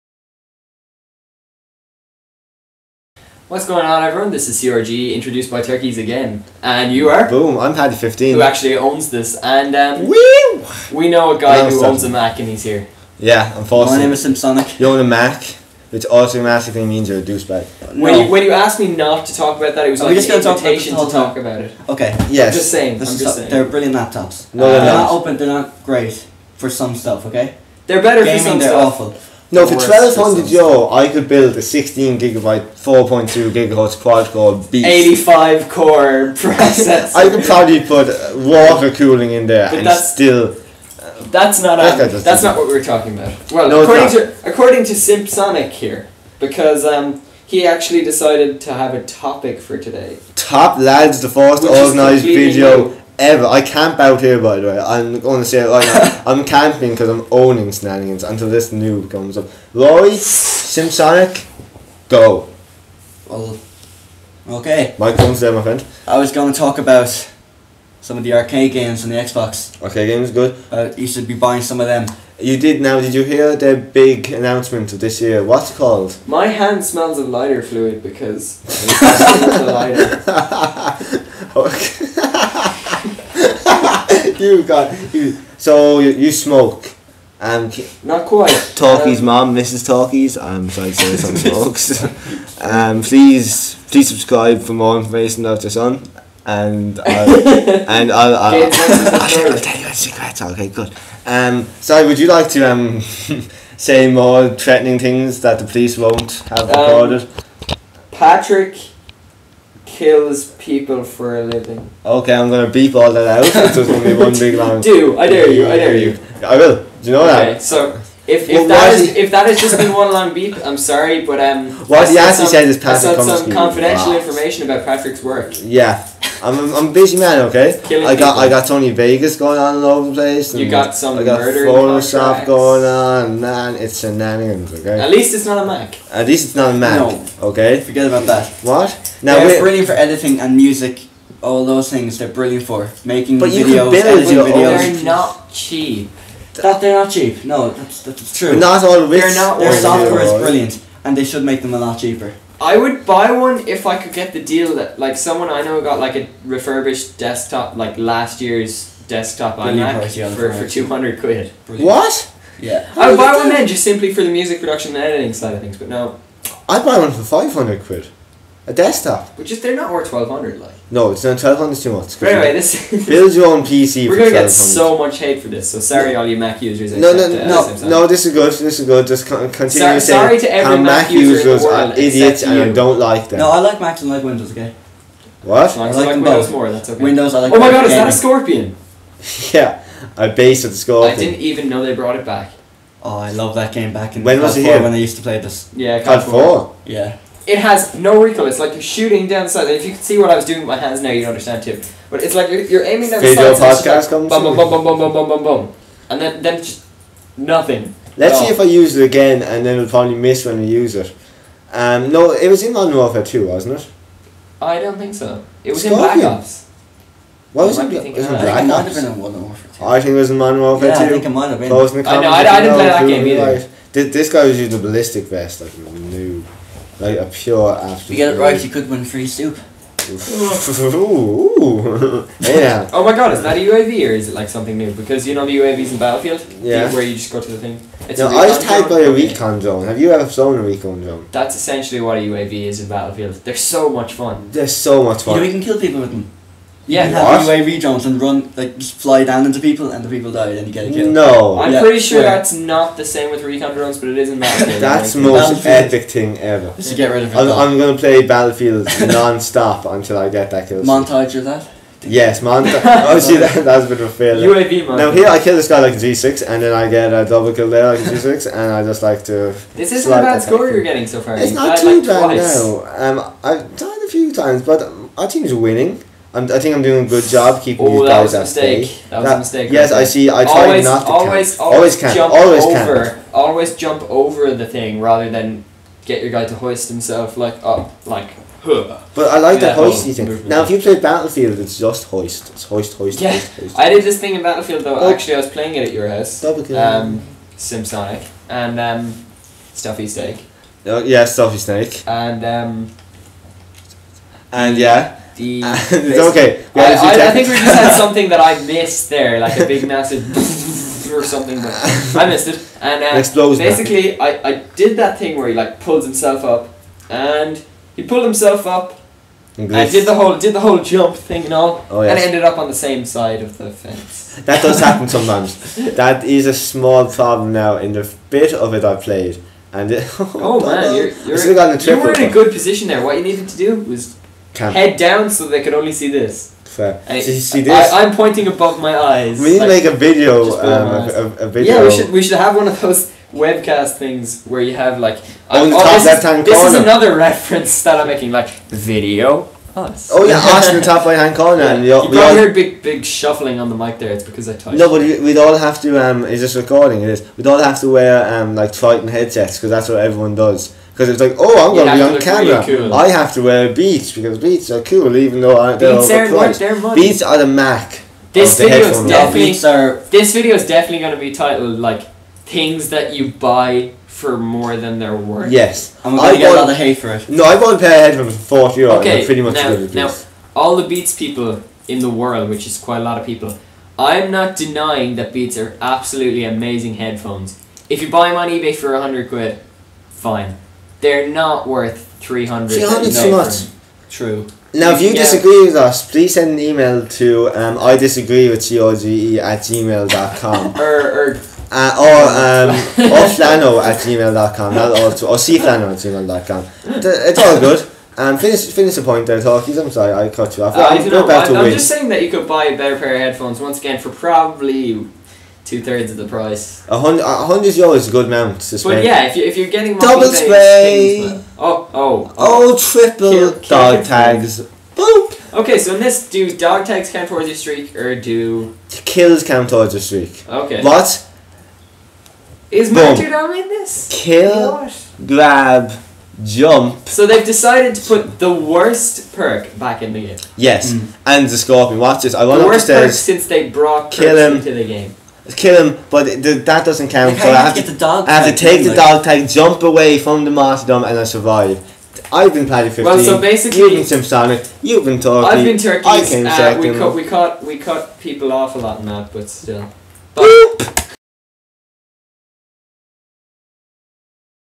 Mac Guy talks crap
CRGE explode onto your screens with talk about crap macs and what not! PADDY15crge is on the gameplay while turkeeeeez and simpsonic lend vocal support